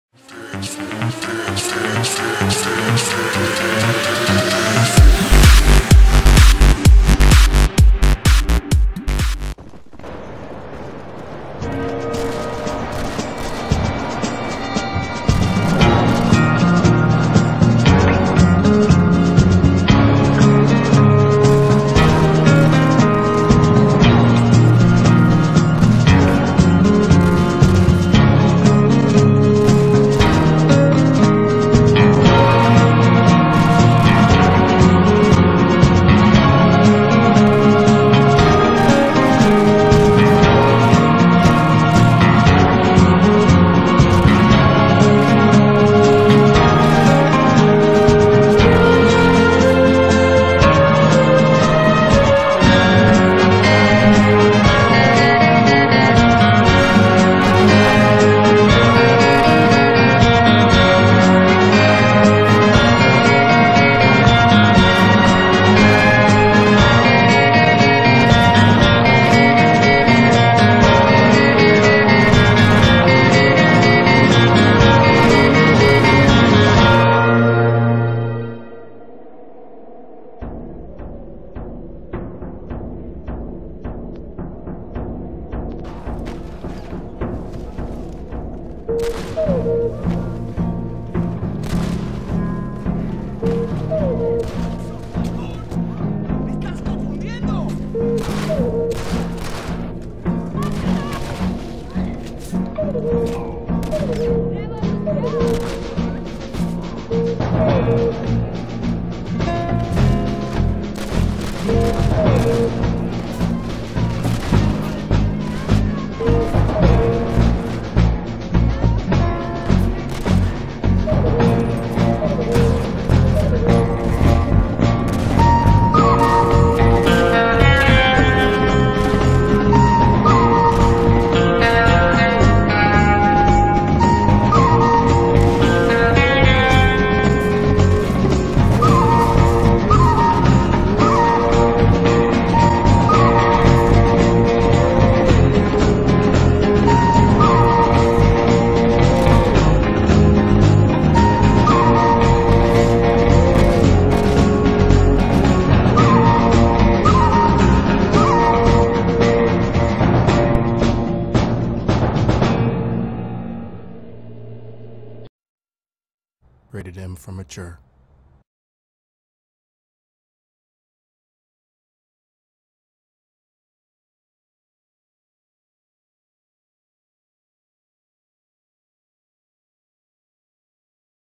GunFighter: Return of Jesse James
Hey it's Lambo, From Lambo Productions(C) bringing you our new fan made video trailer! This is not a real game but it was a really good video game trailer we made. So like it to support us!

-Cheers
Sub,Comment,Like